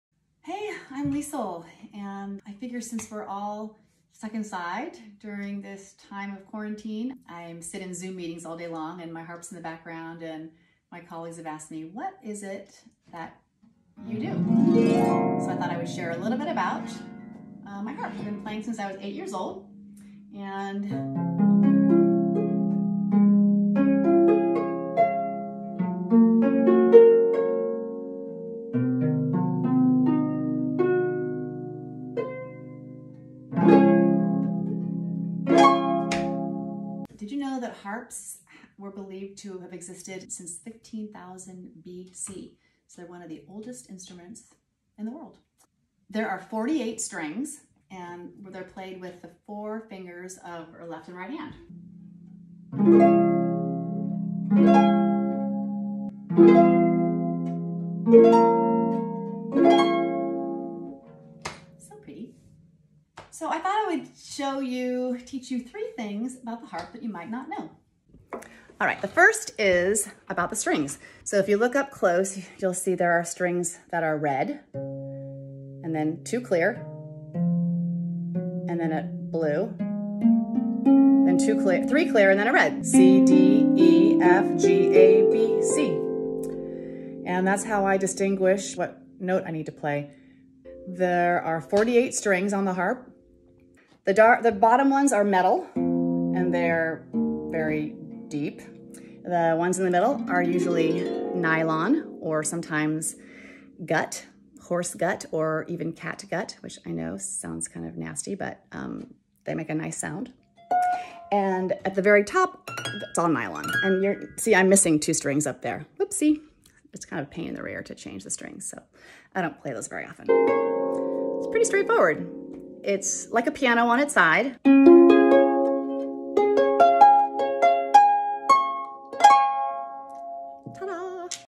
#1 Harp 101 with Liesl - How to tell the strings apart
Have you ever seen a concert harp? There are 48 strings on a concert harp which are red, blue, and clear. The strings are played with four fingers of the right and left hand. Our pinkies are too short to reach the strings. Watch how I tell the strings apart and know what notes to play.